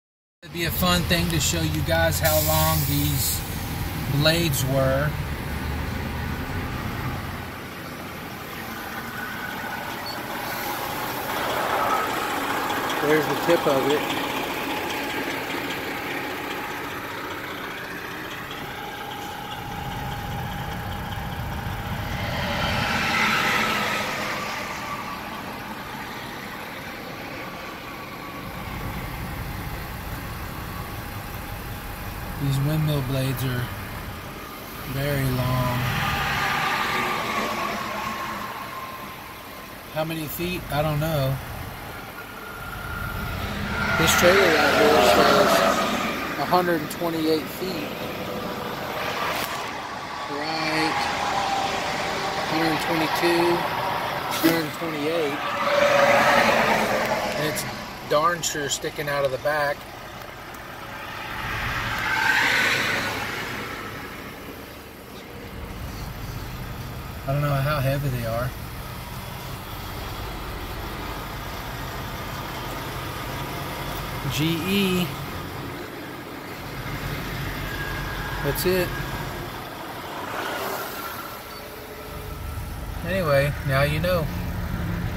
HUGE WINDMILL BLADE ON A TRUCK - Transporting a GE Wind Turbine Rotor
GE 56.9 meter (186.5') wind turbine windmill rotor on a trailer hauled by Peterbilt.

The blade itself is 186 and a half feet. I talk to the driver after this video and he said the truck and the windmill blade together is 215 ft long.

I-40 mile marker 54